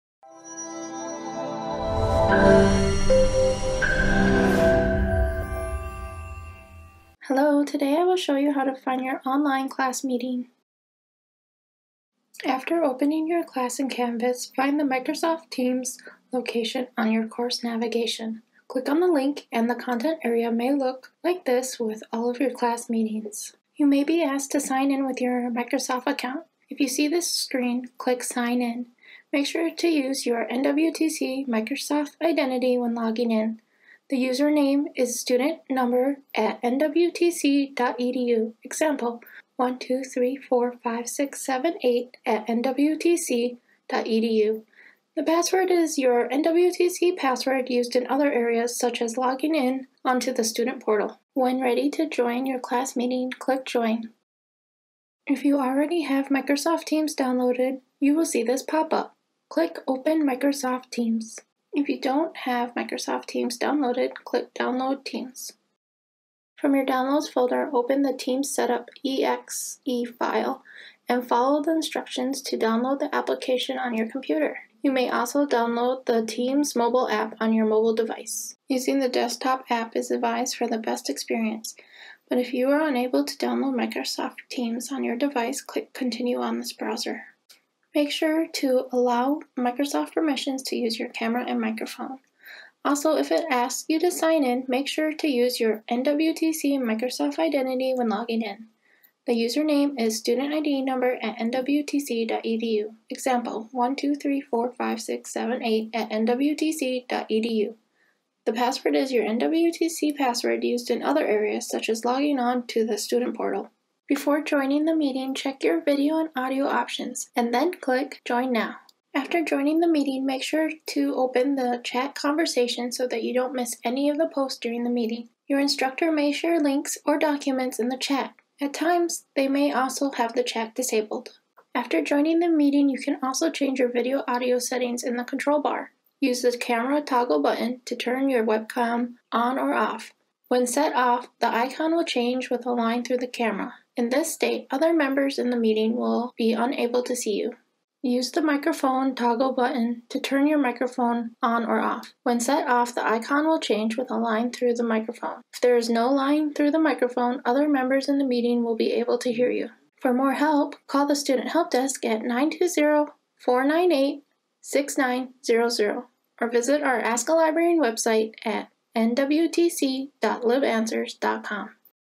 Accessing Web Conference Meetings in Canvas at NWTC
This short video explains how NWTC students can access their Teams meetings through Canvas.